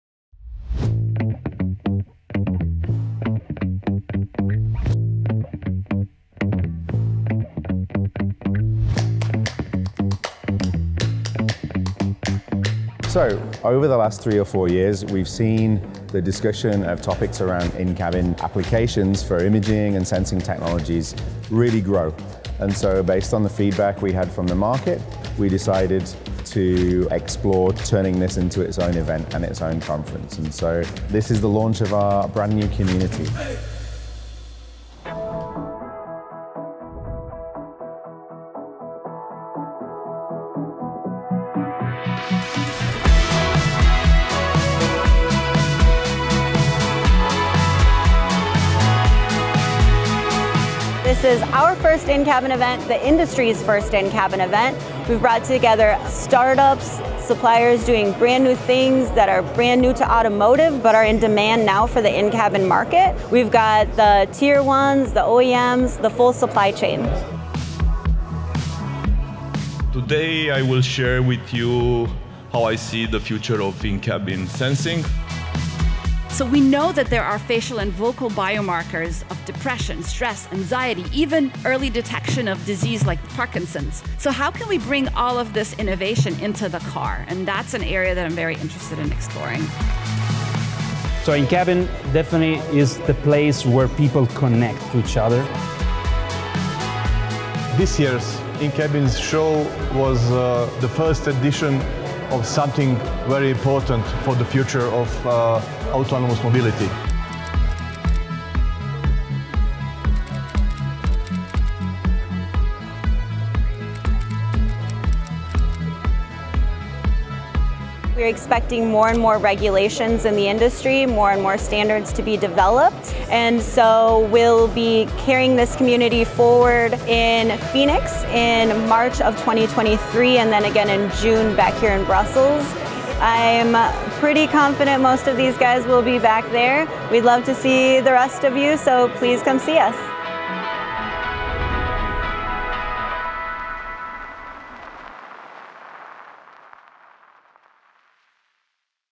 InCabin Brussels 2022 - The Launch of the in-cabin community 🚀
Check out the best moments from our record-breaking launch of InCabin in Brussels! Engineers from across the supply chain joined us from all over the world to discuss, develop and display technology at research at the cutting-edge of intelligent interior systems for automotive.

Tickets for InCabin Pheonix in March 2023 are available, find our more and get your pass

Recorded at InCabin Brussels in 2022, a brand-new Community for automotive in-cabin specialists, held at Autoworld, Brussels.